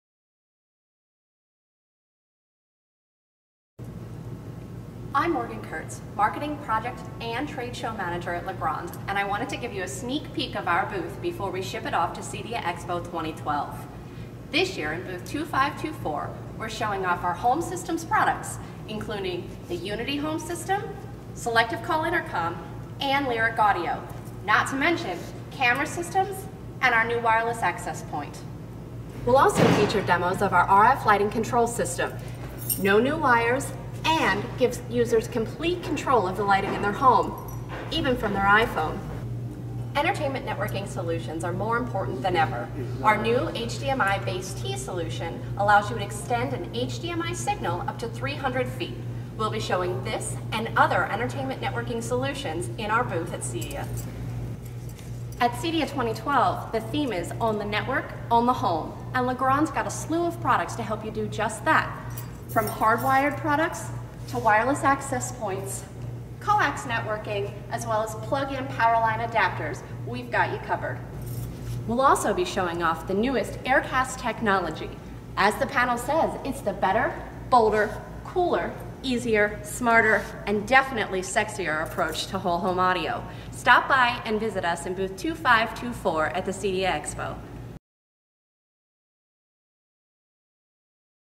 On-Q: CEDIA EXPO 2012 Preview
Get a sneak peek of the On-Q products that we'll be showing in booth number 2524 of the CEDIA Expo.